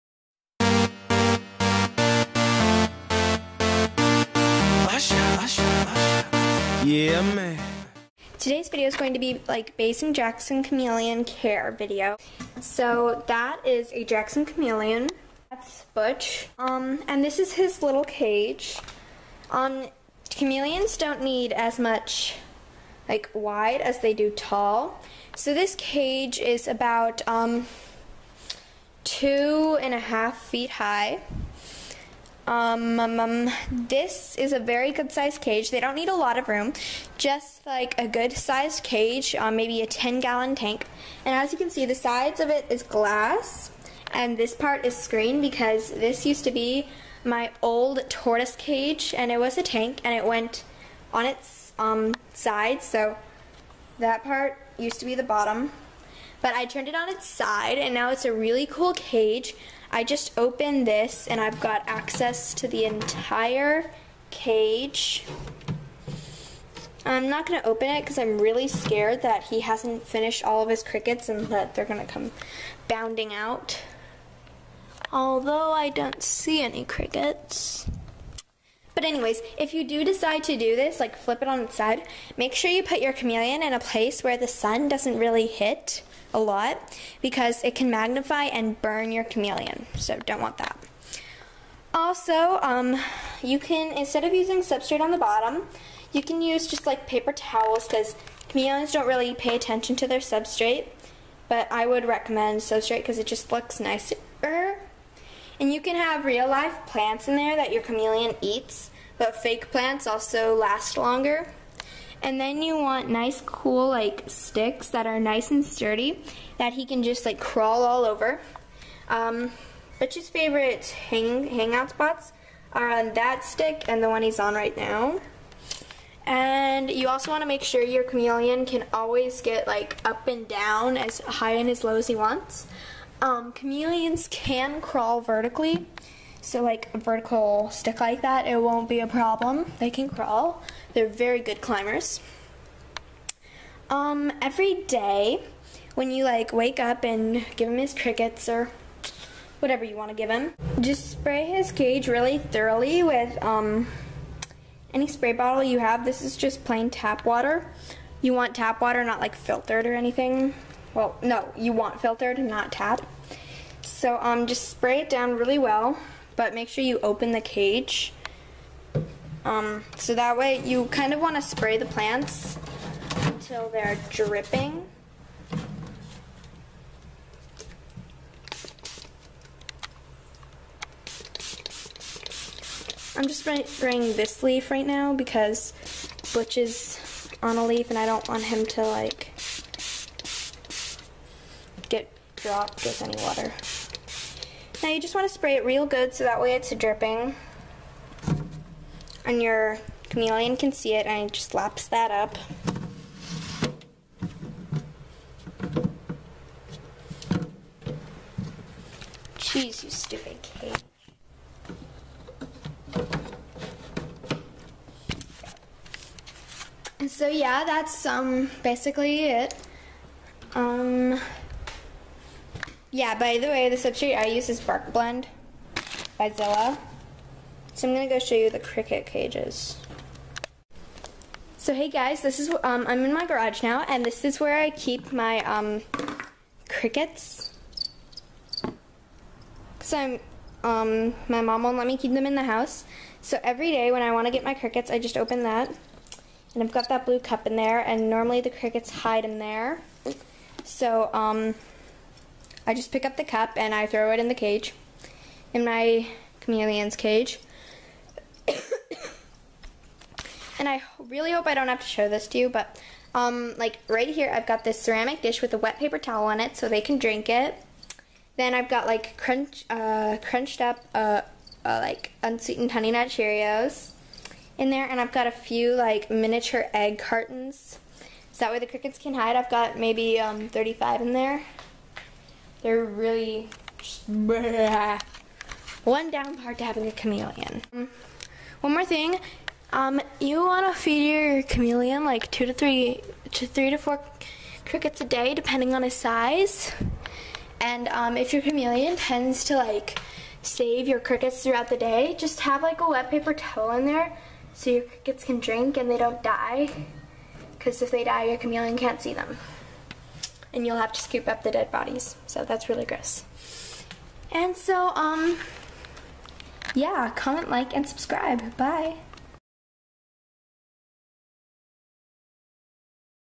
Jackson Chameleon Care
I know I have no UVB lights for him in this video. He was a rescue and I had gotten him on short notice, so thats why I had no UVB, I planned to get some in the future, however I found his original owner.

I hope you enjoyed todays video! I will do my best to answer them!